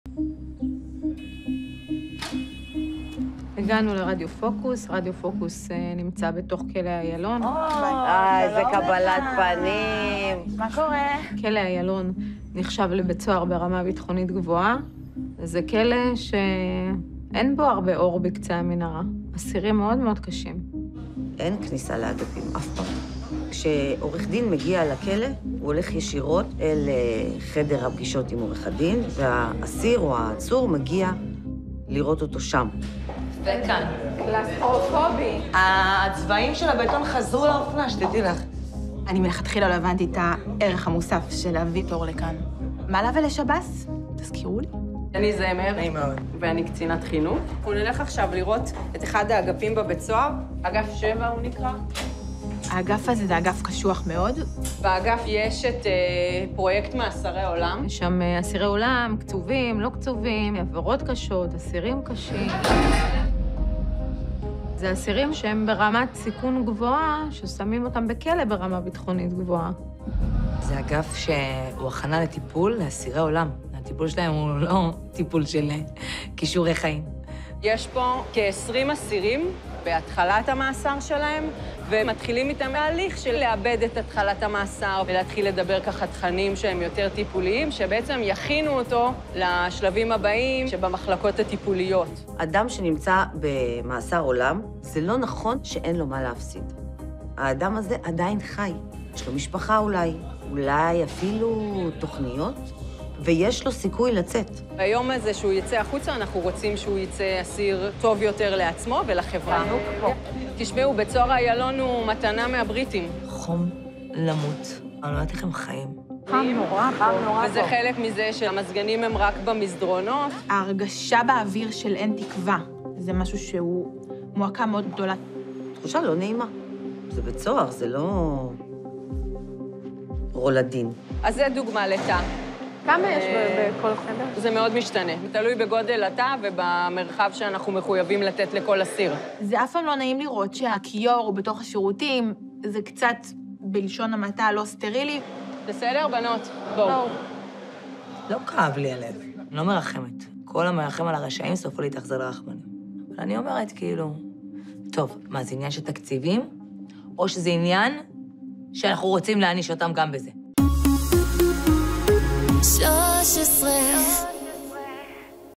"אגף הכנה למאסרי עולם": הפליליסטיות מבקרות בכלא
הפליליסטיות נסעו לבקר באגף 7 שבכלא איילון ("זה כלא שאין בו הרבה אור בקצה המנהרה, יש שם עבירות מאוד קשות"). קצינת החינוך שליוותה אותם הסבירה על הטיפולים שהאסירים עוברים: "ביום שהאסיר יוצא החוצה, אנחנו רוצים שהוא ייצא אסיר טוב יותר לעצמו ולחברה"